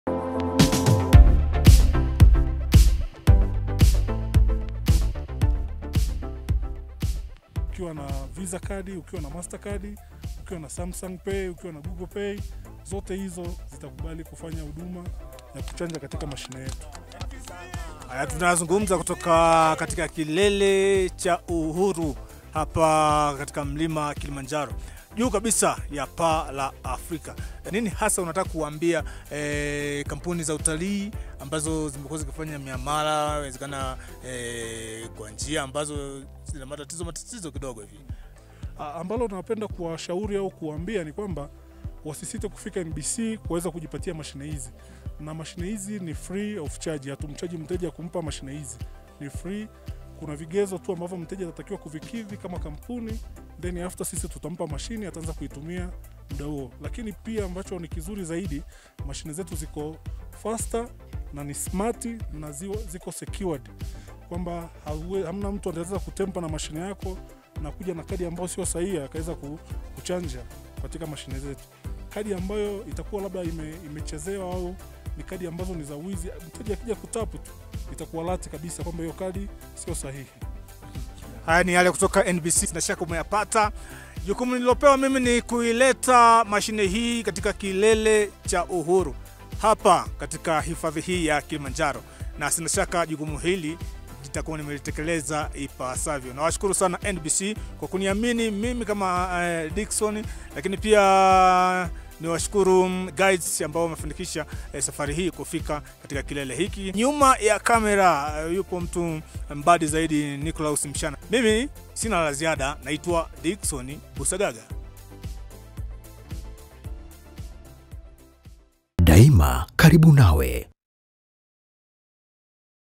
NBC Tanzania | Benki ya kwanza kufanya muamala kupitia mashine ya POS juu ya Mlima Kilimanjaro
Tumeandika Historia mpya: Benki ya kwanza kufanya muamala kupitia mashine ya POS juu ya Mlima Kilimanjaro ⛰️ Mita 5,895/ Futi 19,341.

Tumefanikiwa kufika kilele cha Uhuru. Hatua hii inaakisi uhuru wa kufanya miamala  pia tunadhihirisha umahiri wa teknolojia tuliyokuwa nayo katika utoaji wa huduma zetu kwa wateja hasa kupitia mashine zetu za POS.

Mashine yetu ni ya Kijanja yenye uwezo wa juu, salama kwa miamala yote, na inapokea kadi zote pamoja na waleti za kidijitali kama Google Pay, Apple Pay, Samsung Pay kwa ku Tap tu kwa kadi, simu janja, au saa janja, halafu muamala unasoma.

Tabasamu Limefika Kileleni!